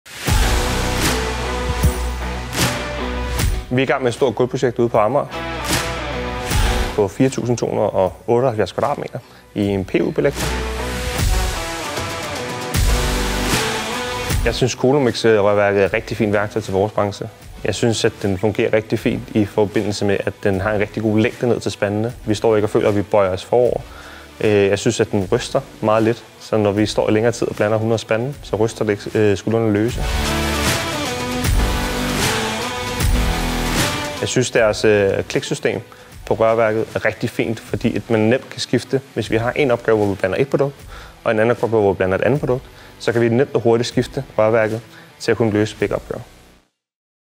NEO COATING prøver Collomix XO1 R "Rigtig fint røreværk til gulvbranchen"
NEO COATING har prøvet Collomix XO 1 R under en af deres store opgaver og de kan tydeligt mærke at Collomix XO 1 R, er et behageligt røreværk at arbejde med.